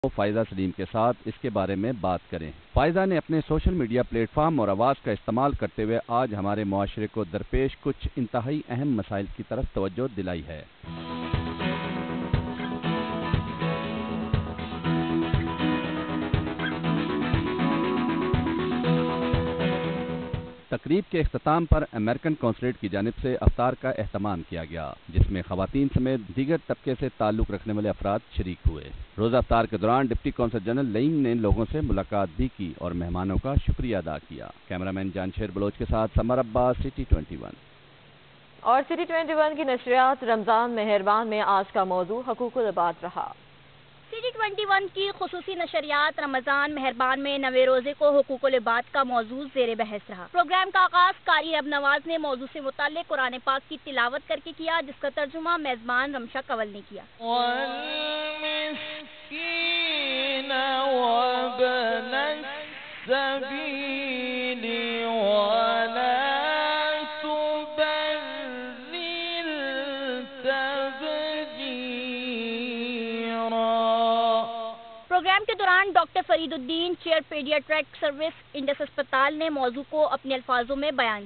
U.S. Consulate Karachi Organized an event on 31st March at PACC
U.S. Consulate Karachi Organized an event on 31st March at PACC (City21 News)